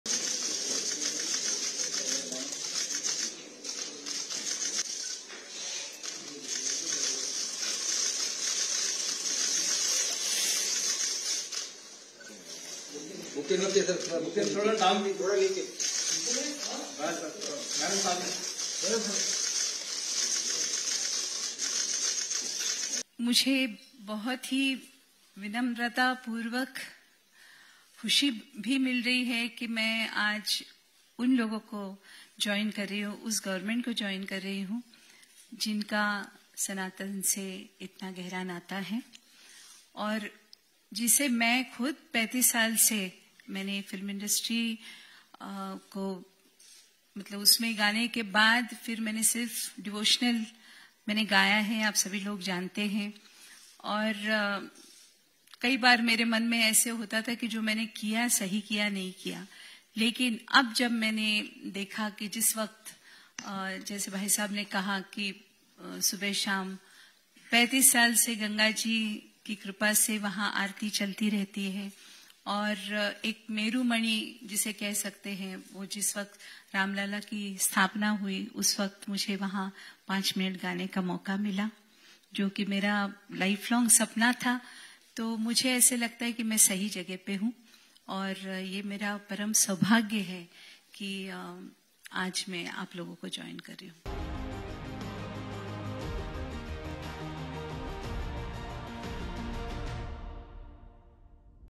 Lok Sabha Chunav : Anuradha Paudwal ने ज्वाइन की बीजेपी, कहा - सनातन से जुड़े लोगों से जुड़ रही हूँ !
अनुराधा पौडवाल (Anuradha Paudwal) की आज राजनीतिक पारी की शुरुआत हुई। अनुराधा ((Anuradha) आज भाजपा में शामिल हो गईं। वो ऐसे समय में भाजपा (Bjp)में शामिल हुईं जब कुछ ही देर में चुनाव आयोग (Election Commision) लोकसभा चुनाव की घोषणा कर सकता है। चुनाव आयोग आज आम चुनावों की तारीखों के साथ कुछ राज्यों के विधानसभा चुनाव की भी घोषणा कर सकता है।